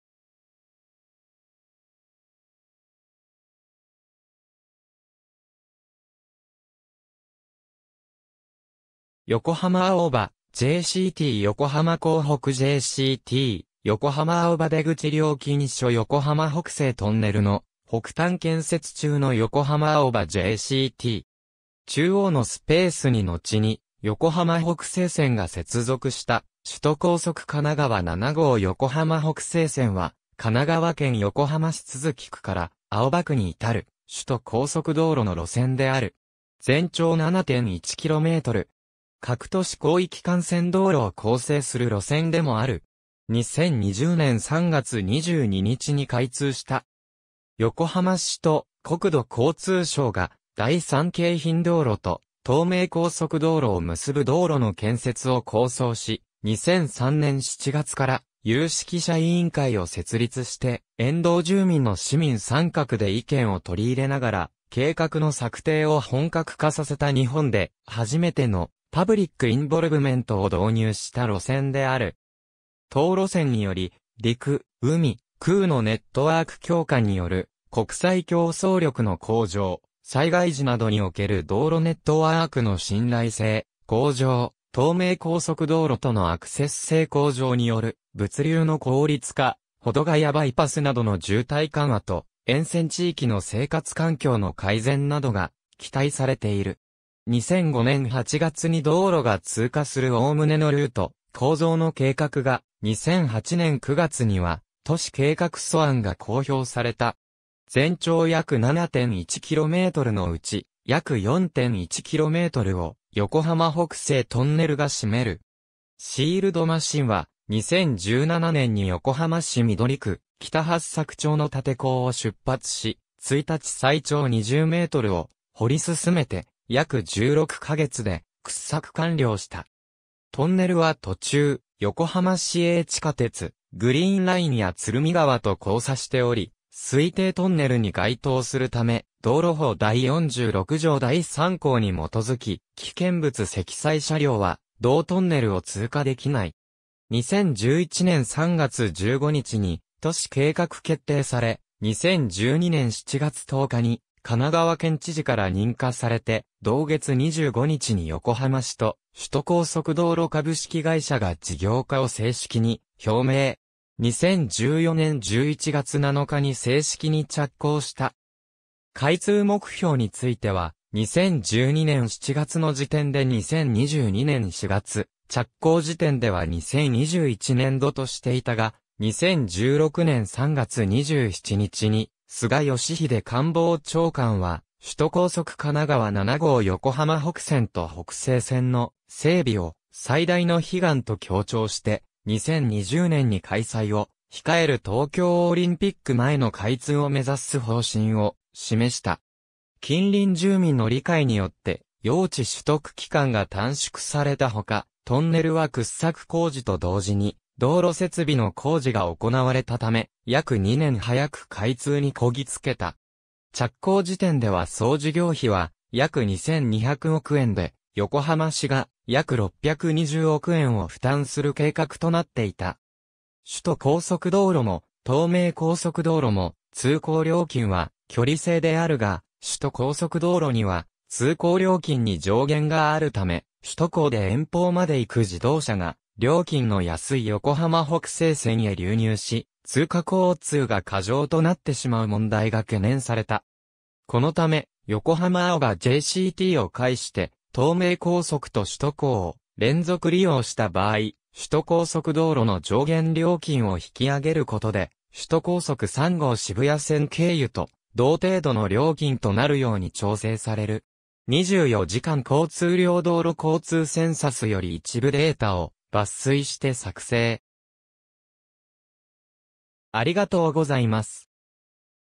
首都高速神奈川7号横浜北西線
横浜青葉JCT（右下方向が北西線、2020年4月） 横浜港北JCT（左上が北西線、2020年4月） 横浜青葉出口料金所（2020年4月） 横浜北西トンネルの北端（2020年4月） 建設中の横浜青葉JCT。
中央のスペースにのちに横浜北西線が接続した（料金所真下より撮影、2018年5月） 首都高速神奈川7号横浜北西線（しゅとこうそくかながわ7ごうよこはまほくせいせん）は、神奈川県横浜市都筑区から青葉区に至る首都高速道路の路線である。
全長7.1キロメートル (km) 。
核都市広域幹線道路を構成する路線でもある。
2020年（令和2年）3月22日に開通した。
横浜市と国土交通省が「第三京浜道路」と「東名高速道路」を結ぶ道路の建設を構想し、2003年（平成15年）7月から有識者委員会を設立して、沿道住民の市民参画で意見を取り入れながら、計画の策定を本格化させた日本で初めての「パブリック・インボルブメント（Public Involvement, PI）」を導入した路線である。
当路線により、陸・海・空のネットワーク強化による国際競争力の向上、災害時などにおける道路ネットワークの信頼性向上、東名高速道路とのアクセス性向上による物流の効率化、保土ヶ谷バイパスなどの渋滞緩和と沿線地域の生活環境の改善などが期待されている。
2005年（平成17年）8月に道路が通過する概ねのルート・構造の計画が、2008年（平成20年）9月には都市計画素案が公表された。
全長約7.1 kmのうち、約4.1 kmを「横浜北西トンネル」が占める。
シールドマシンは、2017年に横浜市緑区北八朔町の立て坑を出発し、1日最長20メートル (m)を掘り進めて、約16カ月で掘削完了した。
トンネルは途中、横浜市営地下鉄グリーンラインや鶴見川と交差しており、水底トンネルに該当するため、道路法第46条第3項に基づき危険物積載車両は同トンネル（事実上本路線全線）を通過できない。
2011年（平成23年）3月15日に都市計画決定され、2012年（平成24年）7月10日に神奈川県知事から認可されて、同月25日に横浜市と首都高速道路株式会社が事業化を正式に表明。
2014年（平成26年）11月7日に正式に着工した。
開通目標については、2012年（平成24年）7月の時点で2022年（令和4年）4月、着工時点では2021年（令和3年）度としていたが、2016年（平成28年）3月27日に菅義偉官房長官は、首都高速神奈川7号横浜北線と北西線の整備を「最大の悲願」と強調して、2020年（令和2年）に開催を控える東京オリンピック前の開通を目指す方針を示した。
近隣住民の理解によって用地取得期間が短縮されたほか、トンネルは掘削工事と同時に道路設備の工事が行われたため、約2年早く開通にこぎ着けた。
着工時点では総事業費は約2,200億円で、横浜市が約620億円を負担する計画となっていた。
首都高速道路も東名高速道路も、通行料金は距離制であるが、首都高速道路には通行料金に上限があるため、首都高で遠方まで行く自動車が、料金の安い横浜北西線へ流入し、通過交通が過剰となってしまう問題が懸念された。
このため、横浜青葉JCTを介して東名高速と首都高を連続利用した場合、首都高速道路の上限料金を引き上げることで、首都高速3号渋谷線経由と同程度の料金となるように調整される。
24時間交通量（台）　道路交通センサス （出典：「平成27年度全国道路・街路交通情勢調査」（国土交通省ホームページ）より一部データを抜粋して作成）